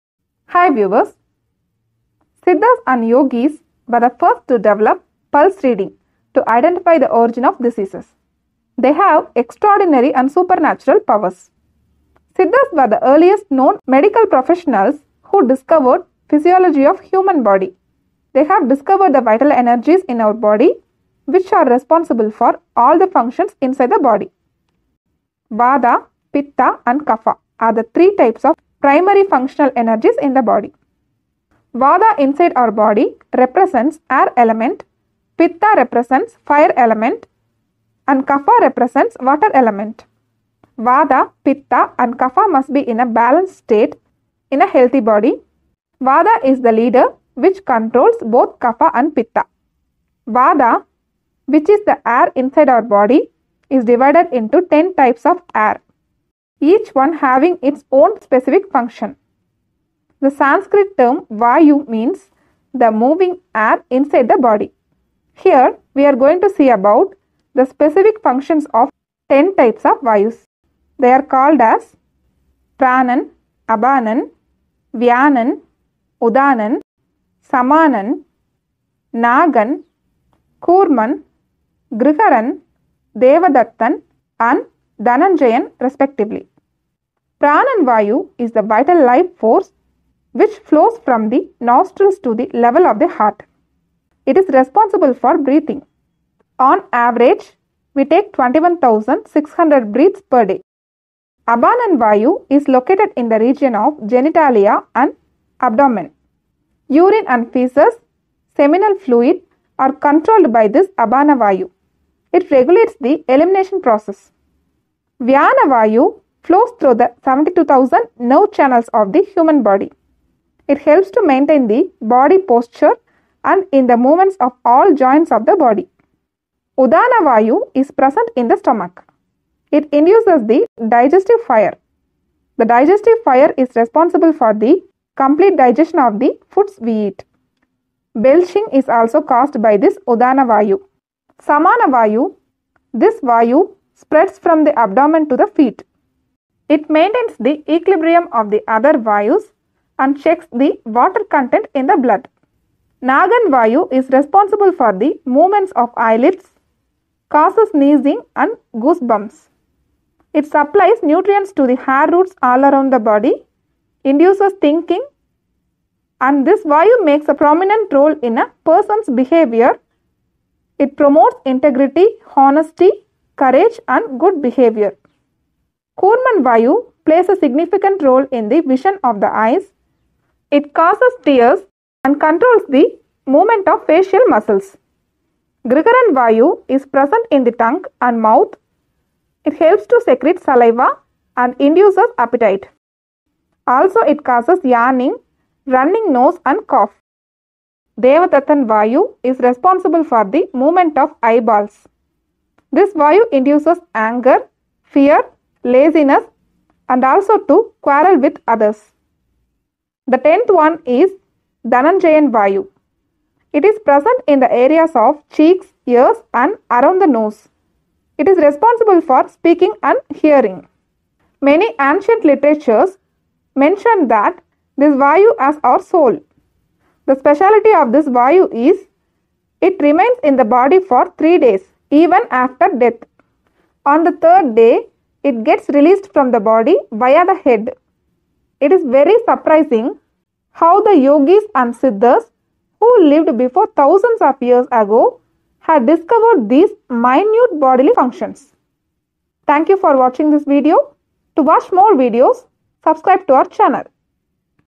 10 types of vayus | Ten types of air inside human body | dasa vayu | vata types | vata dosha
Vata pitta kapha are the three main factors which keeps our body in a healthy state. Vata pitta kapha are the basic essentials during pulse reading in Siddha and Ayurveda system of medicine. vata is of 10 types, called as dasa vayus, each having its own function.